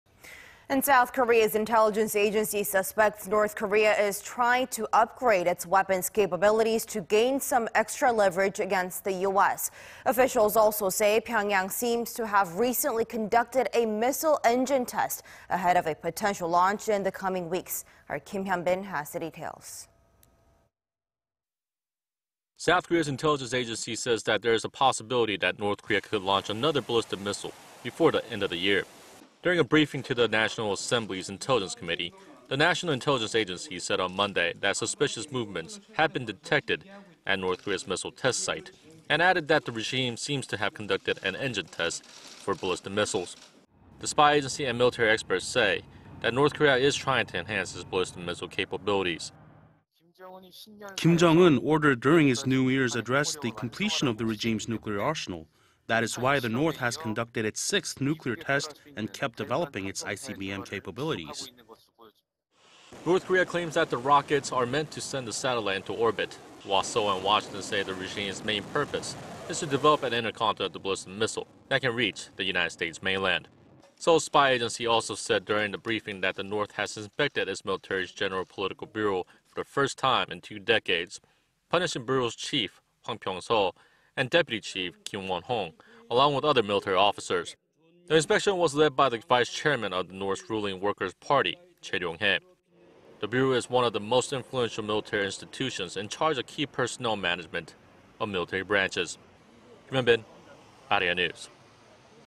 North Korea could launch another ballistic missile before year end: NIS
국정원 "北, 연내 탄도미사일 발사 가능성 주시 중"
  South Korea's intelligence agency suspects North Korea is trying to upgrade its weapons capabilities to gain some extra leverage against the United States.
Officials also say Pyongyang seems to have recently conducted a missile engine test ahead of a potential launch in the coming weeks.

 South Korea's intelligence agency says that there is a possibility that North Korea could launch another ballistic missile before the end of the year.
During a briefing to the National Assembly's intelligence committee, the National Intelligence Agency said on Monday that suspicious movements have been detected at North Korea's missile test site and added that the regime seems to have conducted an engine test for ballistic missiles.
The spy agency and military experts say that North Korea is trying to enhance its ballistic missile capabilities.

(Korean)
   "Kim Jong-un ordered during his new year's address the completion of the regime's nuclear arsenal. That is why the North has conducted its sixth nuclear test and kept developing its ICBM capabilities."

 North Korea claims that the rockets are meant to send a satellite into orbit while Seoul and Washington say the regime's main purpose is to develop an intercontinental ballistic missile that can reach the U.S. mainland.

 Seoul's spy agency also said during the briefing that the North has inspected its military's General Political Bureau for the first time in two decades punishing Bureau's chief Hwang Pyong-so and deputy chief Kim Won-hong along with other military officers.
 The inspection was led by the vice chairman of the North's ruling Worker's Party Choe Ryong-hae.
The bureau is one of the most influential military institutions in charge of key personnel management of military branches.